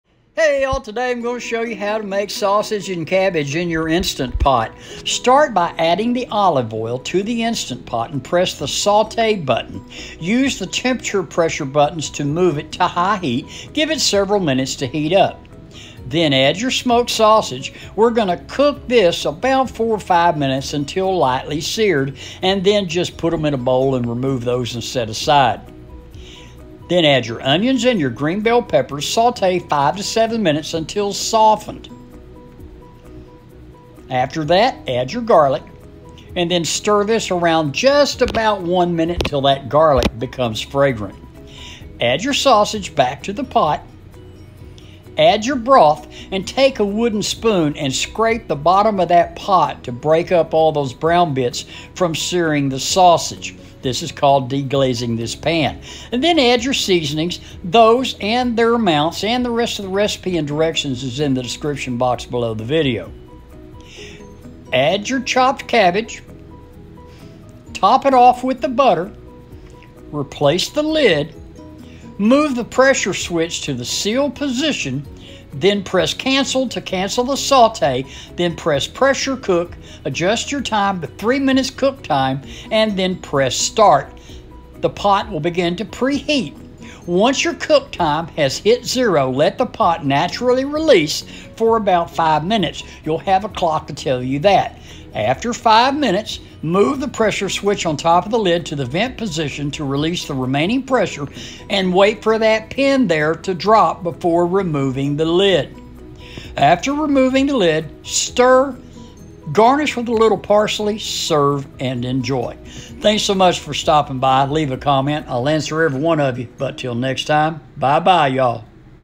INSTANT POT SAUSAGE AND CABBAGE
Instant pot sausage and cabbage is quick and easy one-pot dish with cabbage, onions, peppers.  The perfect meal for any night of the week.

Shopping List:
2 tbsps. olive oil
1 (12) ounce pkg. smoked sausage, sliced into about 1/4 inch rounds
1 large sweet onion, sliced thin
1 small green bell pepper, diced
3 cloves minced garlic
1 small head green cabbage
1 cup chicken broth or stock
2 tsps. kosher salt (1/2 that much if using table salt)
1 tsp. course black pepper
1/4 tsp. red pepper flakes
1/2 tsp. paprika
1/2 tsp. dried oregano
3 tbsps. salted butter
Italian parsley for garnish.

Directions:
Add the oil to the pot and press the saute button on the IP. Allow several minutes to come to high heat.
Add the sausage and saute, stirring frequently, and cook until lightly seared, about 3-4 minutes. Remove and set aside.
Add the onion and peppers and saute 5-7 minutes until softened.
Add the garlic and stir about 1 minute.
Add the chicken broth. Be sure to scrape the bottom of the IP to deglaze the pot.
Add the seared sausage and the seasonings.  Stir.
Add the cabbage and lightly toss. Top the cabbage with the butter.
Secure the lid and move the pressure switch to the "seal" position.
Press the cancel button to take it off of the saute function.
Press the "pressure cook" button.
Use the +- buttons to select 3 minutes cook time. Press the enter button.
When done cooking, allow the IP to naturally release the pressure for 5 minutes.
After 5 minutes, move the pressure switch to the "vent" position and wait for the pressure valve pin to drop.
When the pin drops, it's safe to remove the lid.
Stir, garnish and serve.

Background Eraser software: available through Apple App, Seller:
Tasnim AhmedFilmora.com/io  (Wonderland)
Thumbnail Image: Phonto, available from Apple Store
Thumbnail Image: Canva.com
Thumbnail Image: Remove.bg
Thumbnail Image: designify
Music I use: Apple Music/Simple